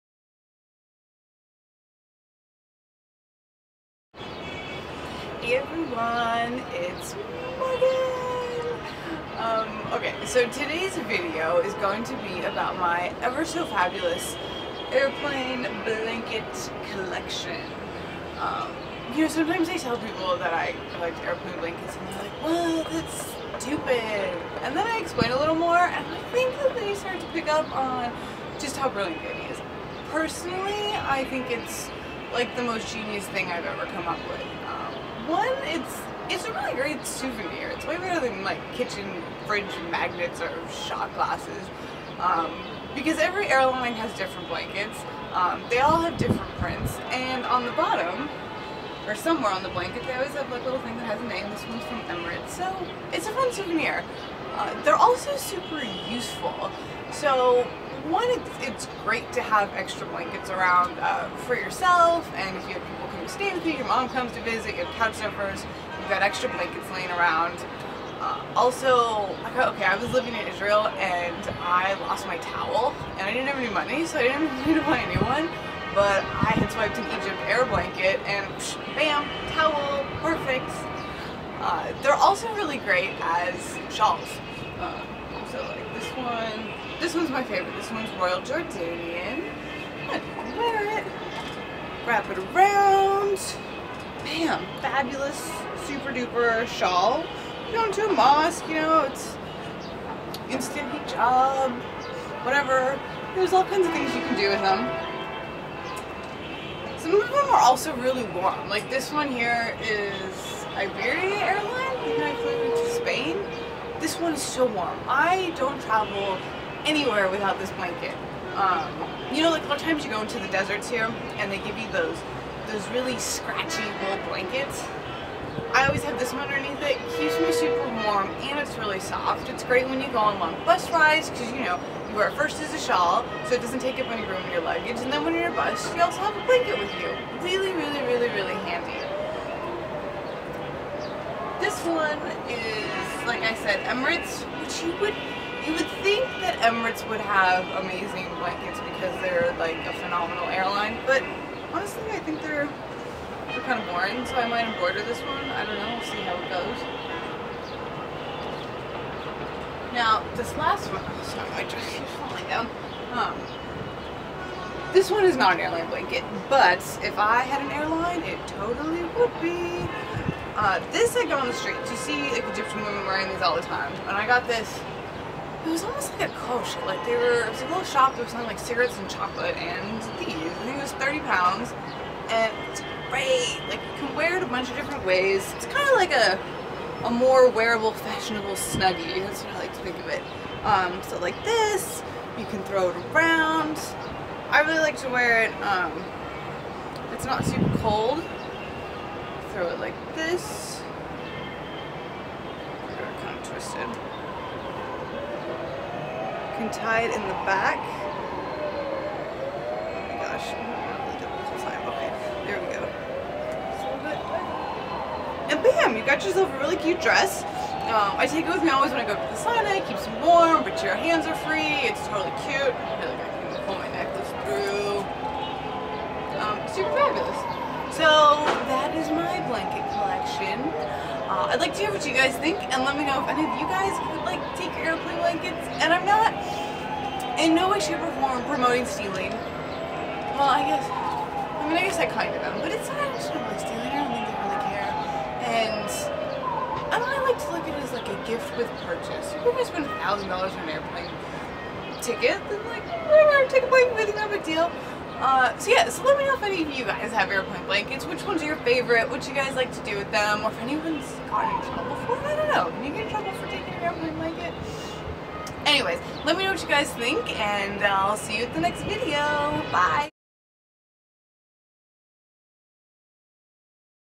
Airplane Blanket Collection
Airplane blankets are my new favorite souvenirs and accessories. I filmed this video in Cairo, Egypt on a houseboat, showing off my favorite blankets and ways to use them. Enjoy :)